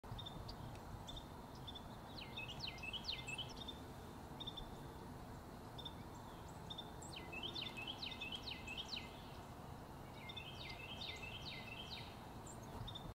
Female deer gives short chase to male deer
I believe these deer are siblings and the female chased the male out of annoyance, but I don’t know.

Two deer eating grass. The female deer chases the male deer.